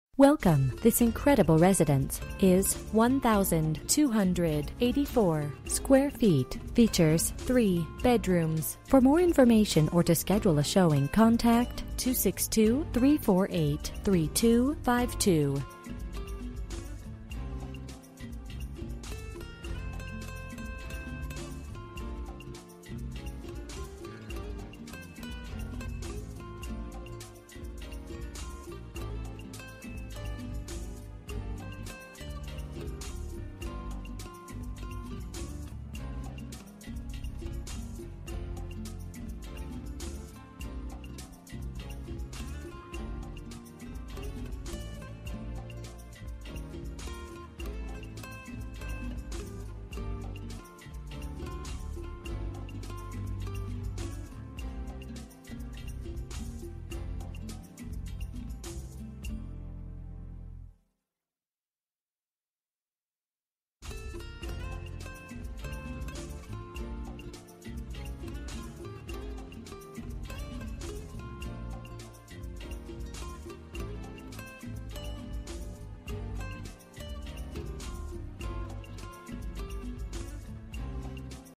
Homes for rent - 1956 E Lakeshore Dr, Twin Lakes, WI 53181
3 bedroom home with access rights to Lake Elizabeth just around the corner. Featuring new flooring throughout, updated kitchen and bath. Newer roof and windows. Beautiful natural wood fireplace. Large deck out back for entertaining and a fenced in backyard that is great for the dog. Come take a look today! Owner will consider base with option and land contract.

Bedrooms: 3
Bathrooms: 0
Square Feet: 1284
Price: $1,200